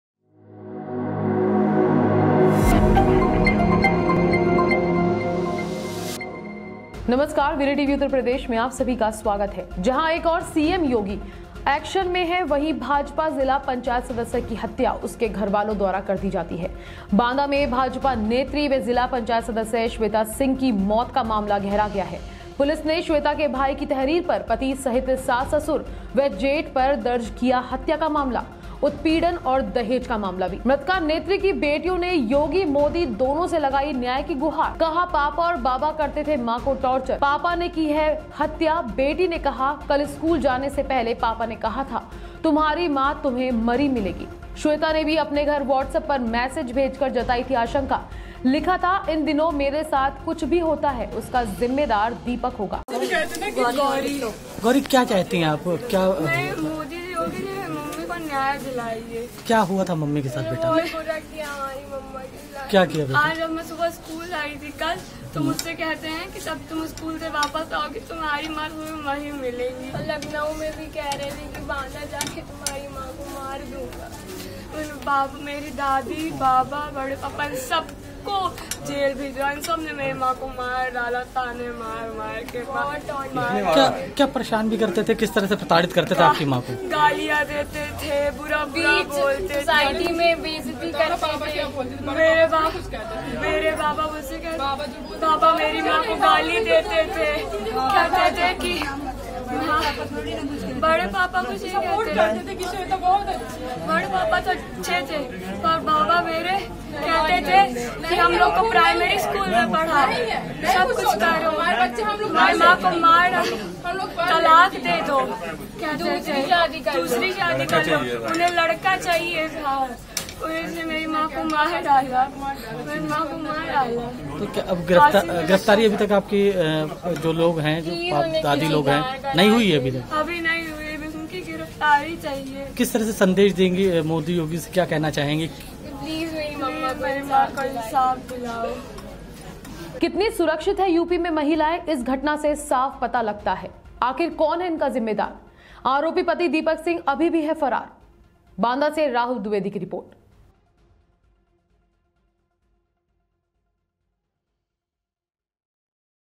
Banda में भाजपा जिला पंचायत सदस्य की हत्या का मामला, पति हुआ फरार | Crime in UP | UP Police
भाजपा नेत्री व जिला पंचायत सदस्य स्वेता सिंह की मौत का मामला,पुलिस ने स्वेता के भाई की तहरीर पर पति सहित सास ससुर व जेठ पर दर्ज किया हत्या, उत्पीड़न व दहेज का मामला,मृतका नेत्री की बेटियों ने योगी मोदी से लगाई न्याय की गुहार,कहा पापा और बाबा करते थे माँ को  टॉर्चर, पापा ने की है हत्या,बेटी ने कहा कल स्कूल जाने से पहले पापा ने कहा था तुम्हारी माँ तुम्हे मरी मिलेगी,स्वेता ने भी अपने घर व्हाट्सएप पर मैसेज भेज कर जताई थी आशंका,लिखा था इन दिनों मेरे साथ कुछ होता है तो उसका जिम्मेदार दीपक होगा,आरोपी पति दीपक सिंह अभी भी है फरार